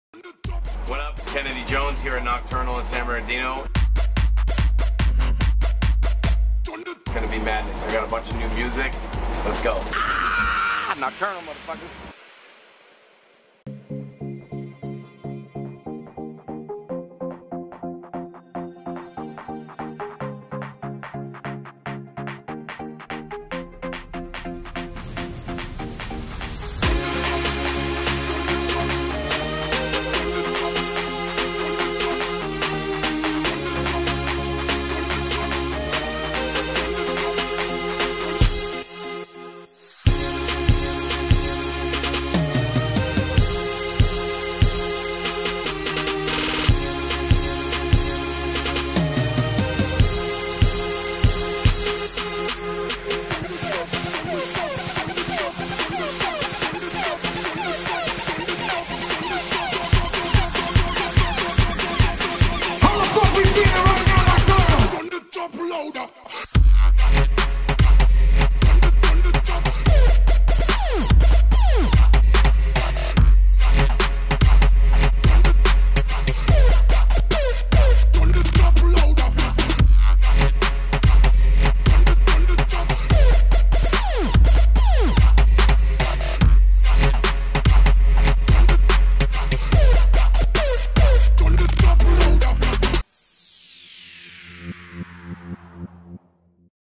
Official Recap Video: Kennedy Jones Nocturnal Wonderland 2014
Here is my official recap video from Nocturnal Wonderland which took place this year at the San Manuel Amphitheater. I had an amazing time playing for so many supporters. Team KJ was definitely in the house!